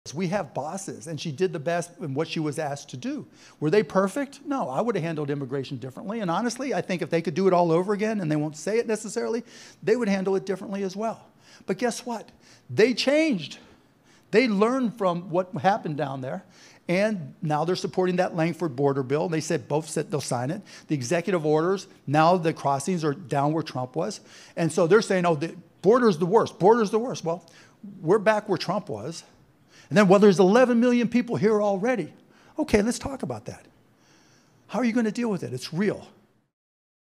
Mark Cuban admits Biden-Harris admin. should have acted ‘sooner’ on border crisis
Mark Cuban admitted Saturday that the Biden-Harris administration should have acted “sooner” to fix the border crisis — while he was stumping for Kamala Harris and Tim Walz in Pittsburgh.

“I would have handled immigration differently,” the business mogul said on the campaign trail on behalf of the Democratic presidential nominee.

“And honestly, I think if they could do it all over again — and they won’t say it necessarily — they would handle it differently as well.”